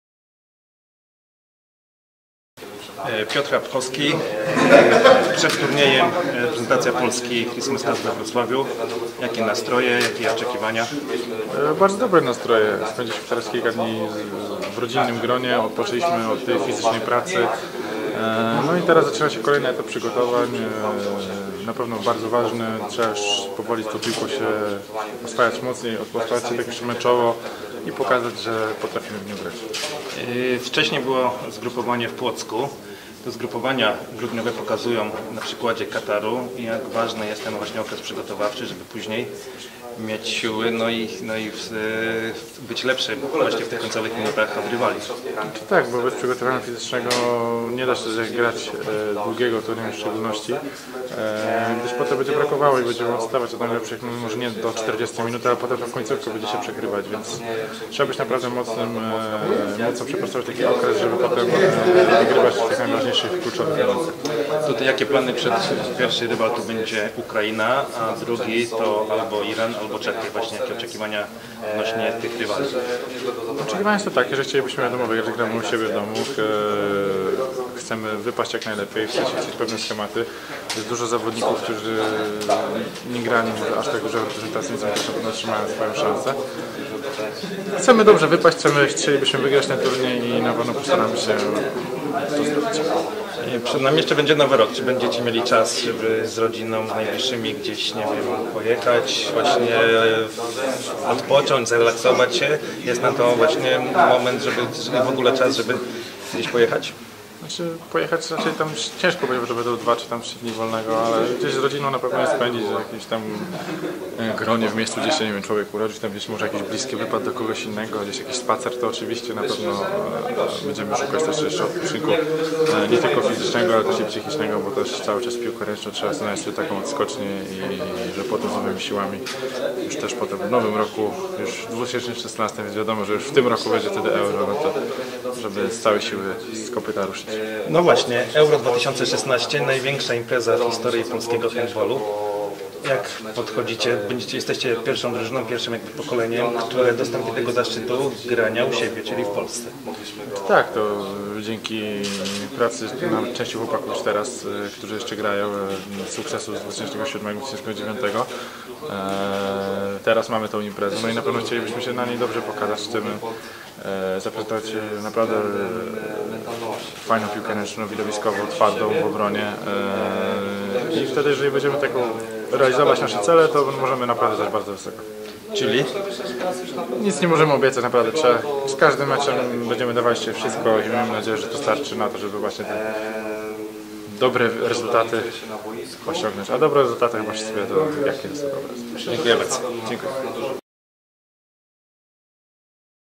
Piotr Chrapkowski: Gramy u siebie
Podczas turnieju we Wrocławiu planujemy przećwiczyć różne warianty gry. Chcemy triumfować w tej imprezie, to dla nas ostatni test przed własną publicznością przed ME. Podczas Euro w każdym meczu będziemy dawali z siebie wszystko, ale teraz nie możemy nic obiecywać, stawka będzie bardzo wyrównana - podkreślił Piotr Chrapkowski przed turniejem Christams Cup we Wrocławiu.